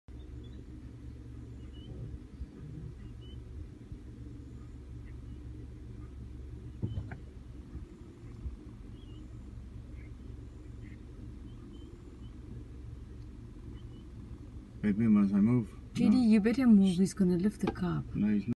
Elephant Bull - take evening stroll, Kruger National Park South Africa
Kruger National Park May 2019. Such a majestic animal, with the same family bonds and ties - I believe soul too, as that of a human. If you haven't been to South Africa change your bucket list. You will not regret. Bring your family get engaged and even married here in this part of Gods land,. Africa the land of beauty and splendour . We will feed you the best you ever had. Sunset fires- watching the stars and listening to the voices and sounds of thousands of insect how they call each other. Birds waking you up early morning with music that the best organ cant play.